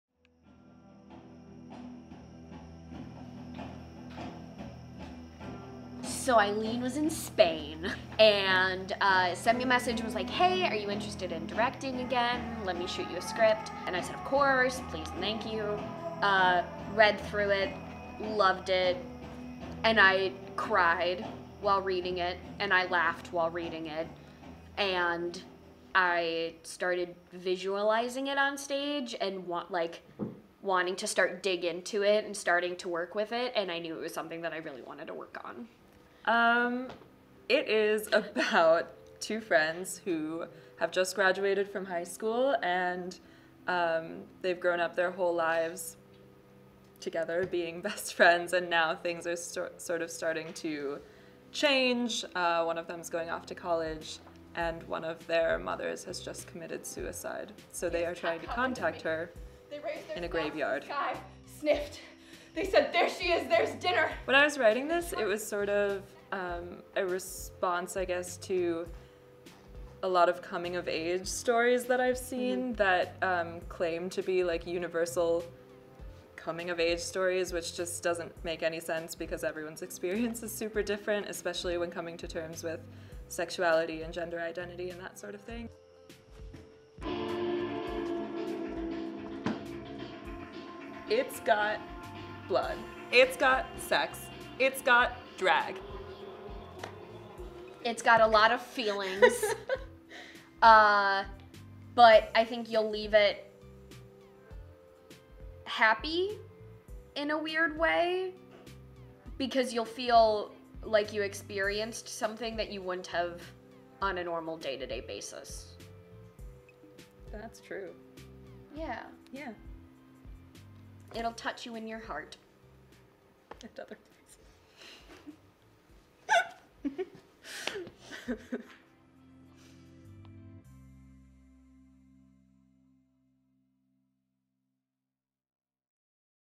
All the Yellows; A contemporary coming of age story
The cast and crew for All the Yellows stage a dress rehearsal on Monday, January 21, 2019. All the Yellows is an original theater piece written, directed, produced, and performed, by current University of Iowa students.

Tate Hildyard